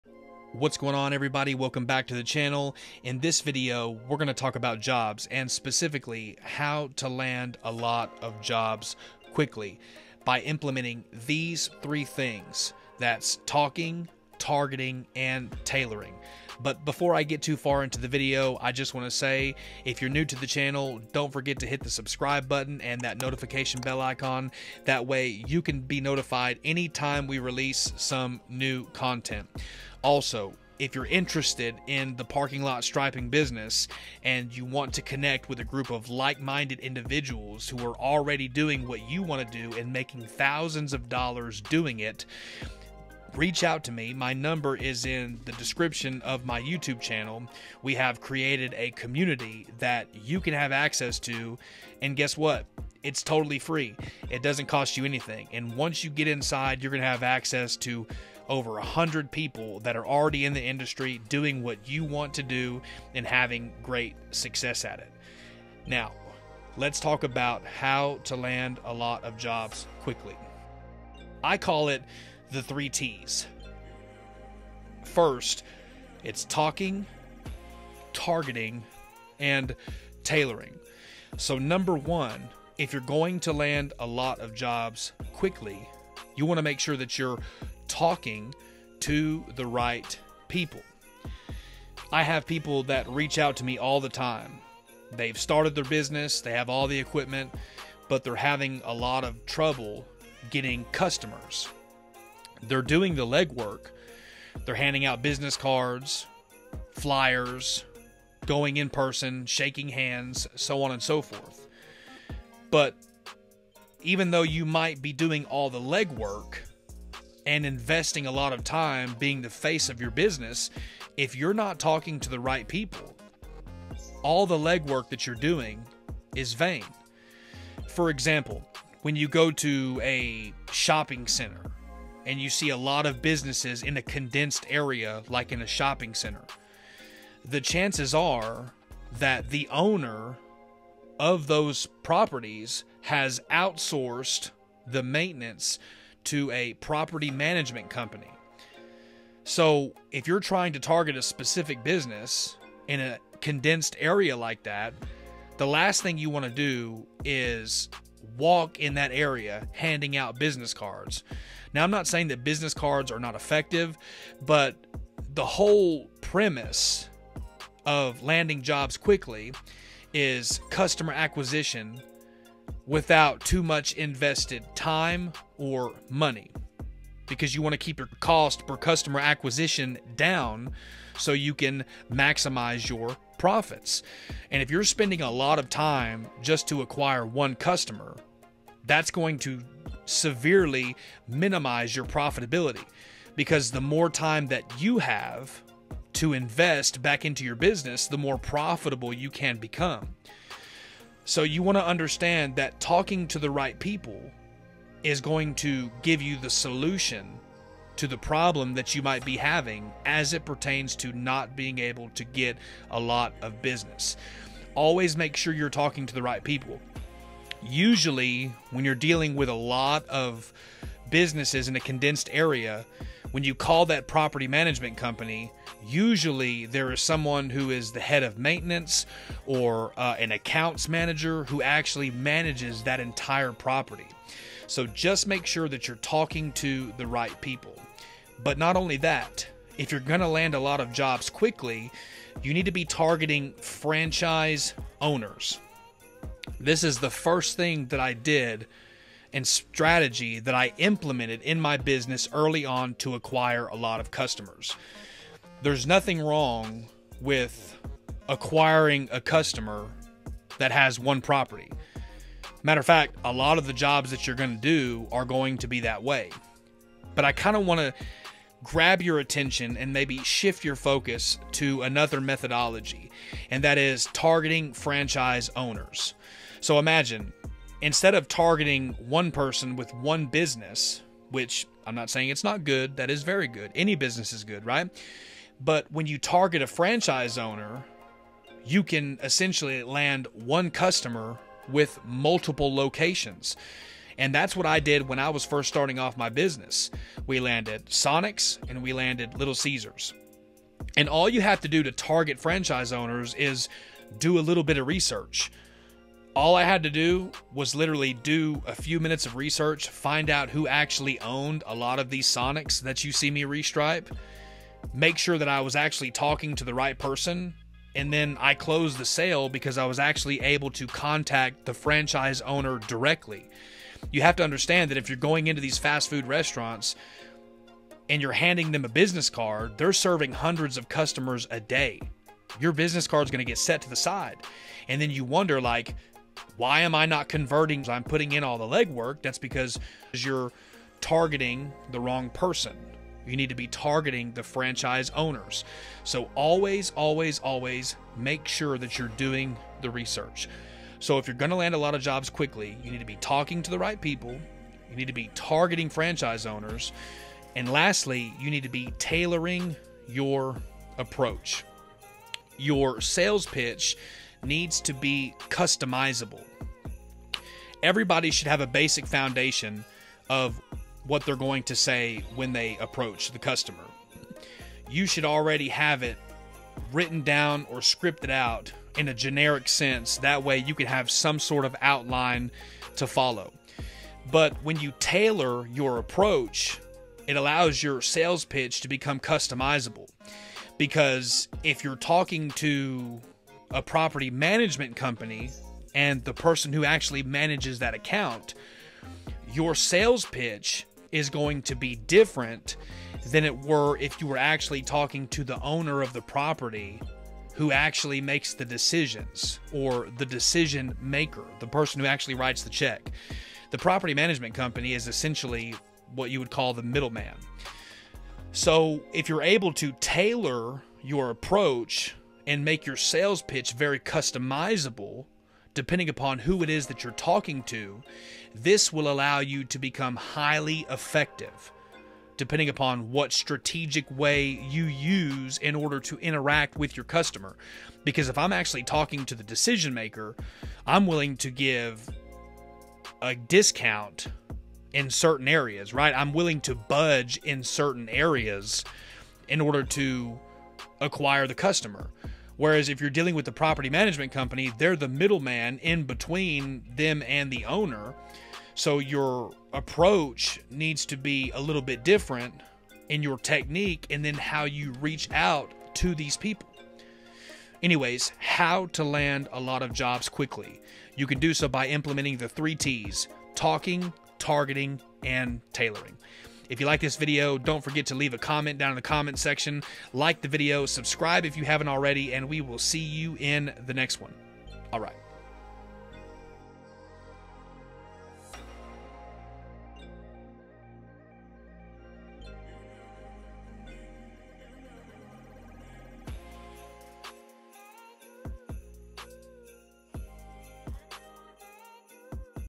HOW to land a lot of JOBS QUICKLY? By implementing the 3(T's) Talking, targeting, and tailoring!!!
Sure! Here’s a YouTube video for acquiring customers for your business by talking, targeting, and tailoring your tactics:

“Looking to boost your business’s customer base? In this video, we dive into the art of acquiring customers by leveraging effective communication, precise targeting, and tailored strategies. Learn how to engage with your audience through compelling conversations that resonate with their needs and desires. Discover the power of understanding your target market and crafting personalized tactics to capture their attention. Whether you’re a small business owner or a marketing professional, these proven techniques will help you attract and retain customers like never before. Join us as we unlock the secrets to successful customer acquisition and watch your business thrive. Don’t miss out on this opportunity to elevate your marketing game!”